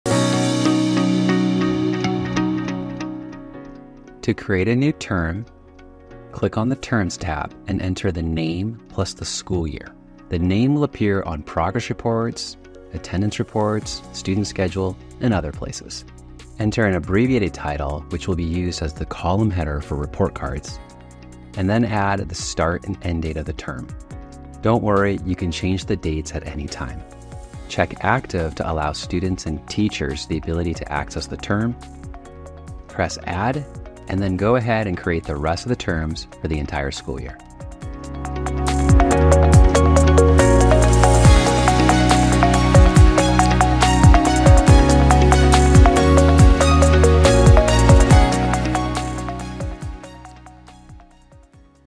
How to Create Quarter 1 in Gradelink – Step-by-Step Setup Tutorial
In this tutorial, learn how to create Quarter 1 in Gradelink and set up a new term for the school year. Follow the easy steps to enter the term name, abbreviated title, and start and end dates. You'll also learn how to activate the term, giving students and teachers access to important features. Plus, we'll show you how to create the remaining terms for the entire school year.

Steps covered in the video:
✅Accessing the Terms Tab
✅Adding the term name, title, and dates
✅Activating the term for student and teacher access
✅Creating all terms for the school year

Get your school year off to a great start with this Gradelink tutorial!